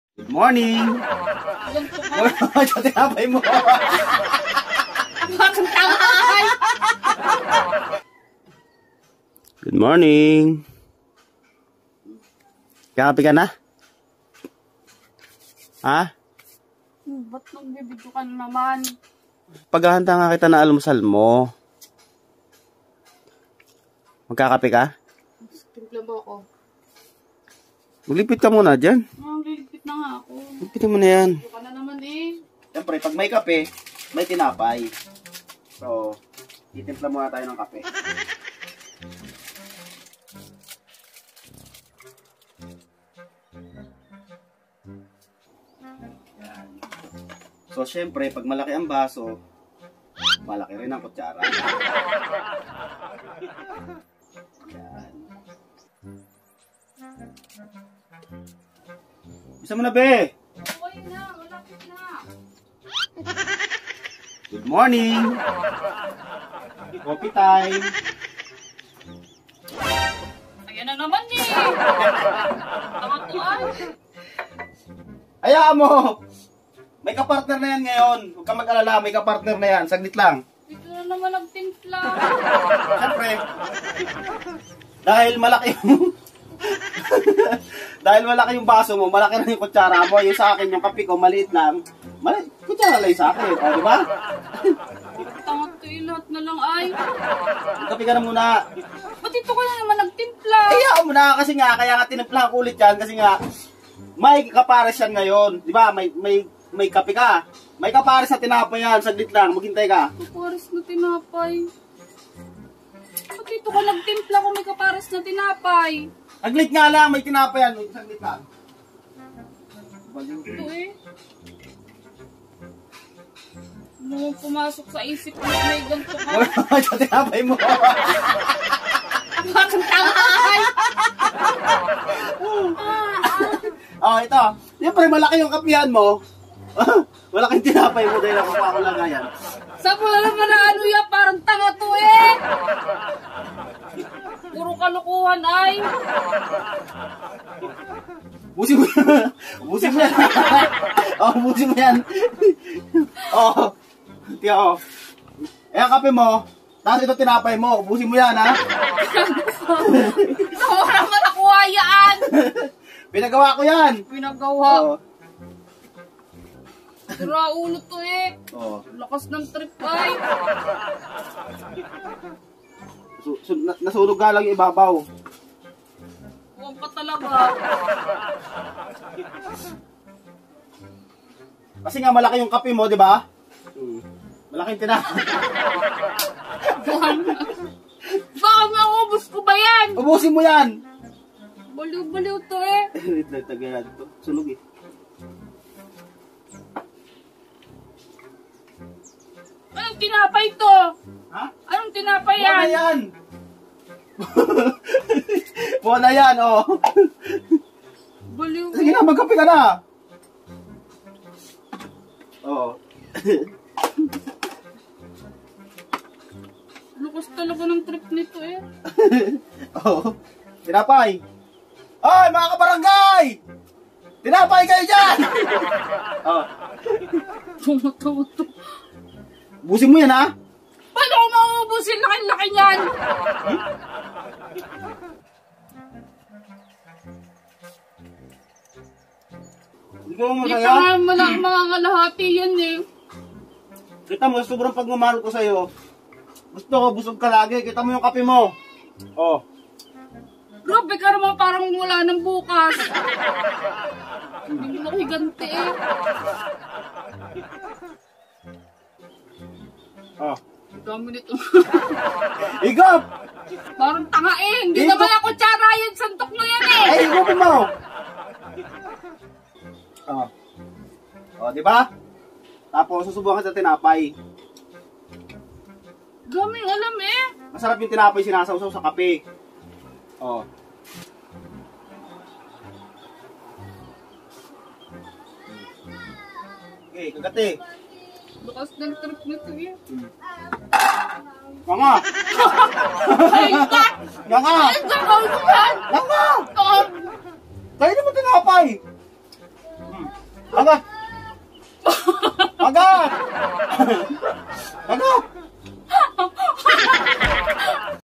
KAPE AT TINAPAY NA PANG BRGY😂 GULAT SI MISIS SA LAKI NG TINAPAY😂
GULAT SI MISIS SA LAKI NG TINAPAY NYA PANG PARES SA MALAKING BASO NG KAPE HAHA😂